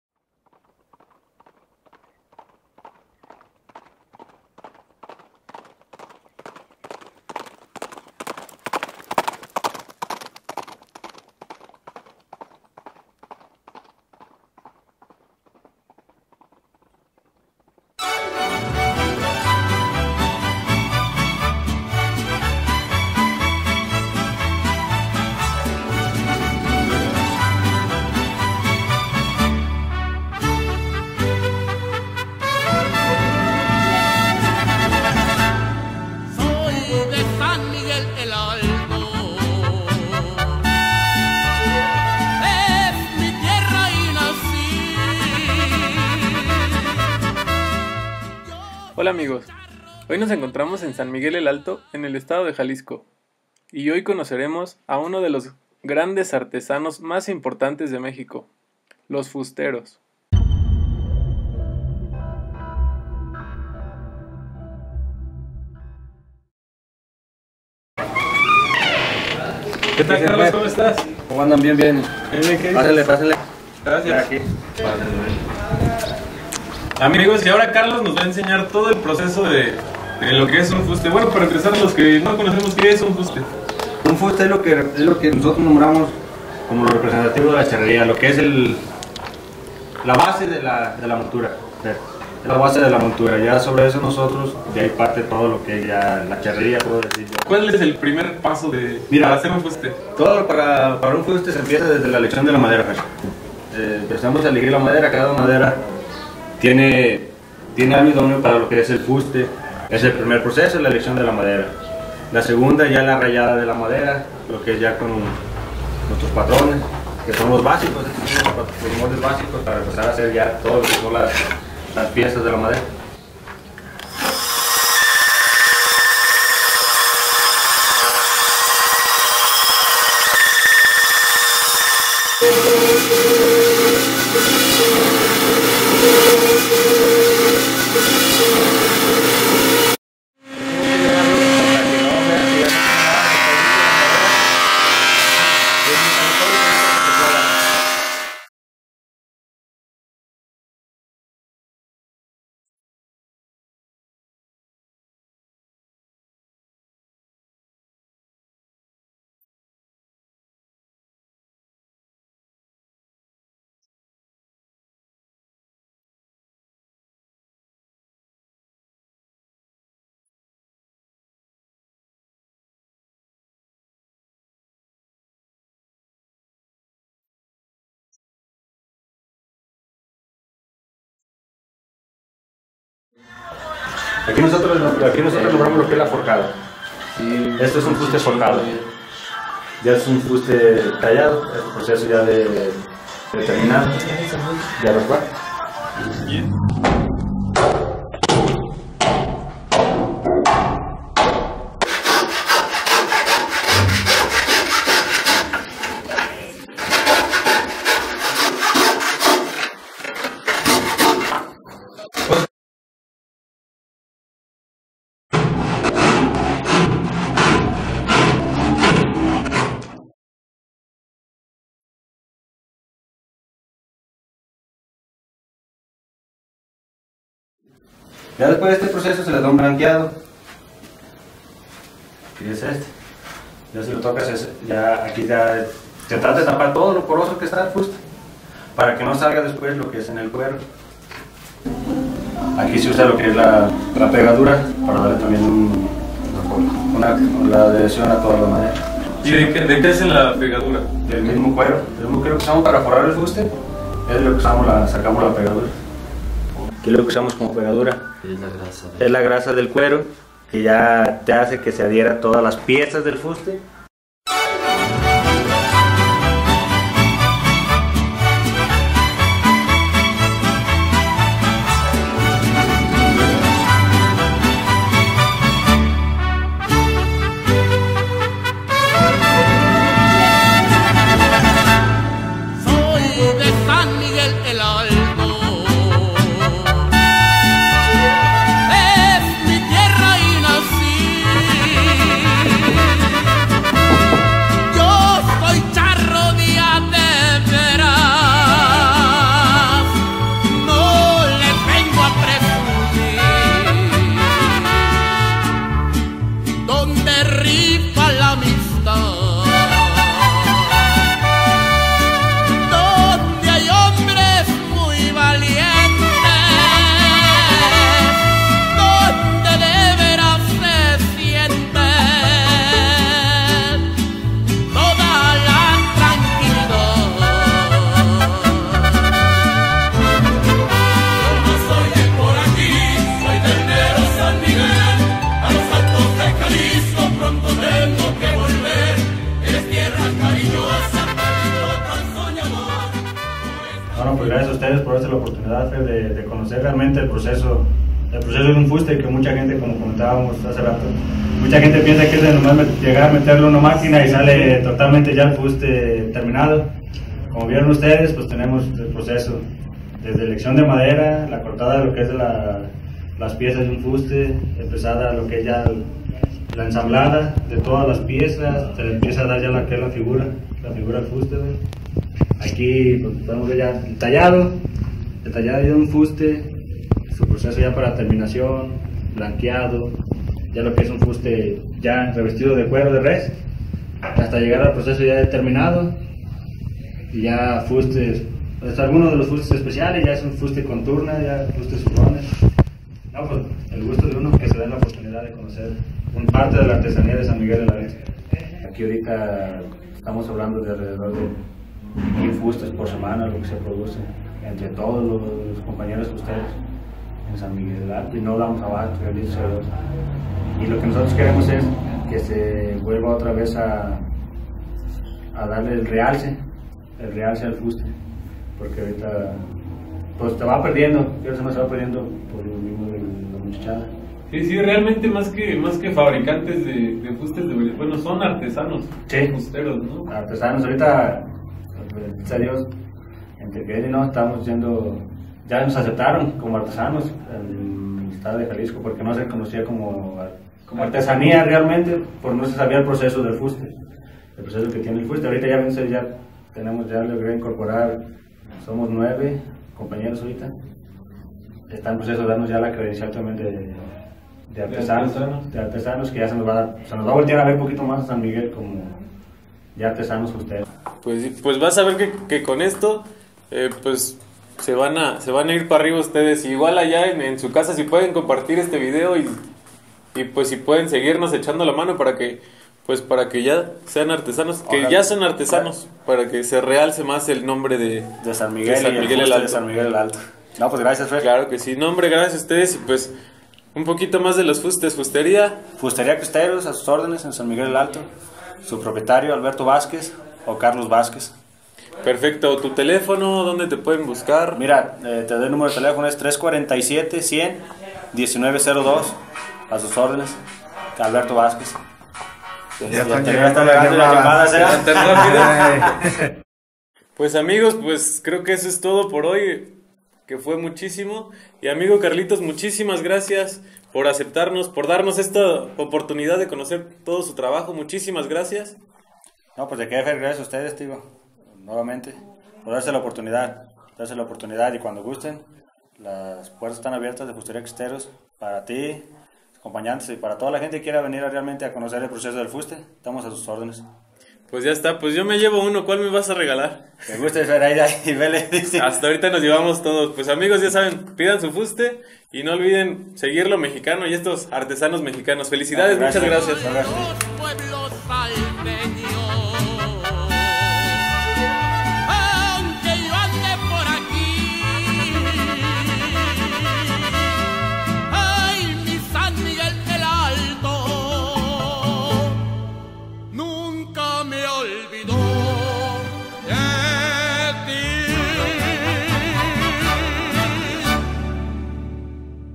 De Caballos con Fer Trujillo. Como se hacen los fustes charros
Fusteros Artesanos Mexicanos
Los artesanos mas importantes para el deporte nacional Mexicano